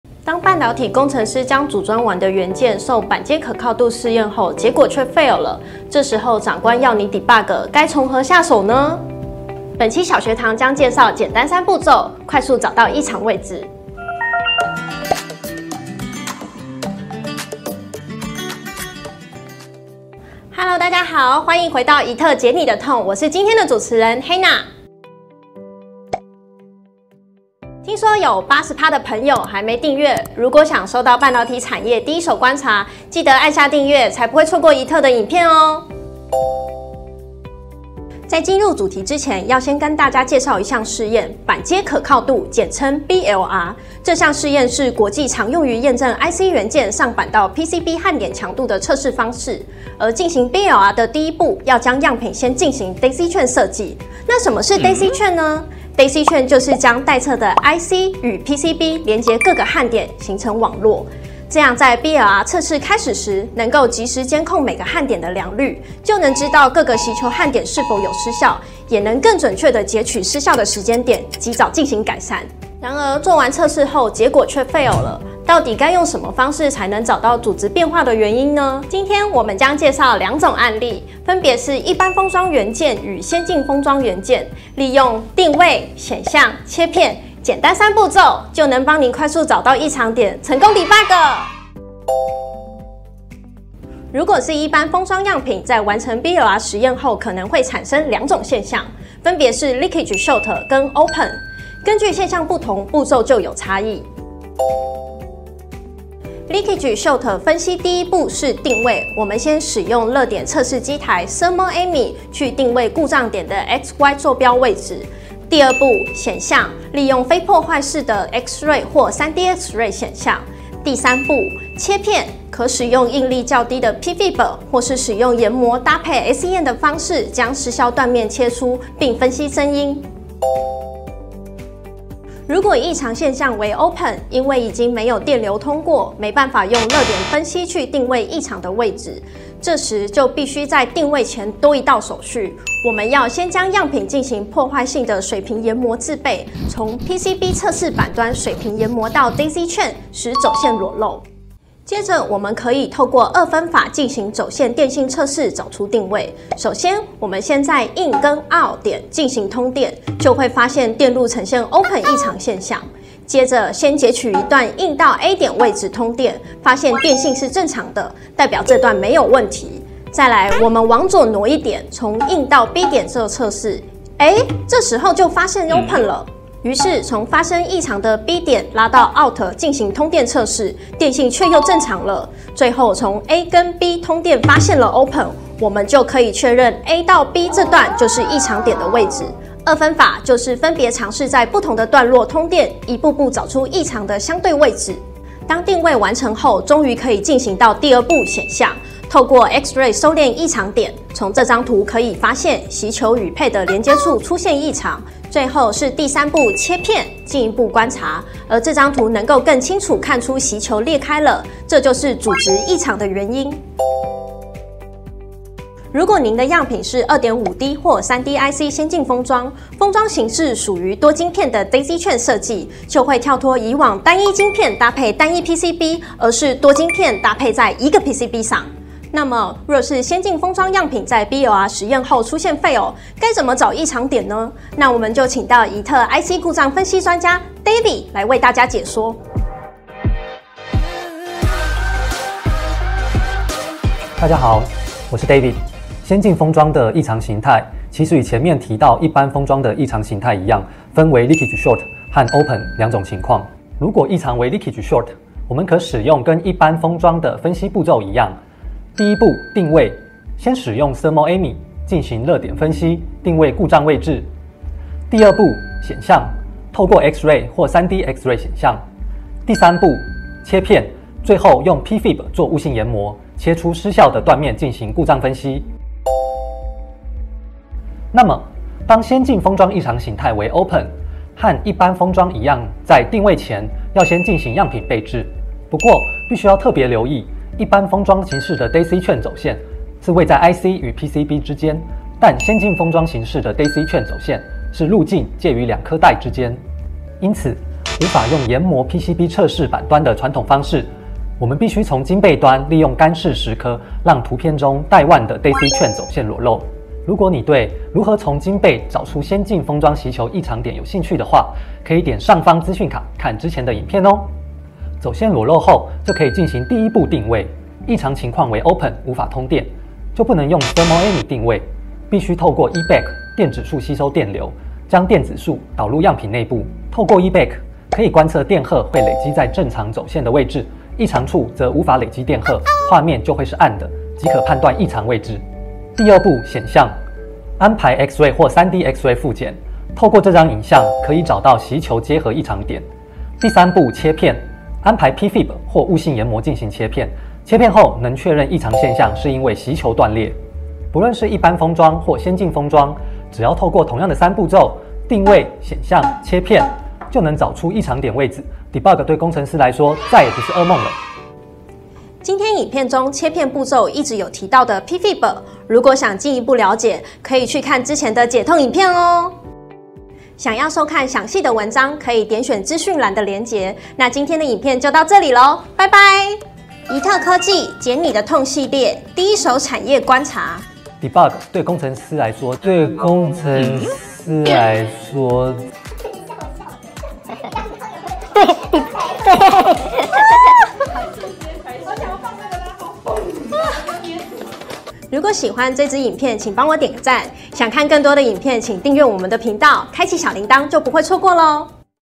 半導體工程師都該看！簡單三步驟 就能找到先進封裝焊點異常【宜特解你的痛 EP.24】
板階可靠度 (BLR)是國際上常用來驗證焊點強度的實驗手法
當實驗結果出爐後卻是Fail 😰，長官要你Debug，這時該如何是好呢🤷‍♀️
別擔心！先判斷樣品是採用 一般封裝元件 還是
宜特小學堂教您用簡單三步驟，快速找出晶片的異常點 ⚠️

00:00 前言
00:20 本集主持人-Hana
00:36 介紹板階可靠度跟菊花鍊
01:25 簡單三步驟
01:32 一般封裝如何找異常點
01:45  一般封裝 Leakage/Short 分析步驟
02:10  一般封裝 OPEN 分析步驟
02:33  介紹二分法
03:55 今日的專家-David
04:06 先進封裝如何找異常點
04:14 先進封裝 Leakage/Short 分析步驟
04:36 先進封裝 OPEN 分析步驟
06:02 主持人結尾
06:21 花絮

📍【相關文章】：
板階可靠度試驗後Fail 如何找先進封裝焊點異常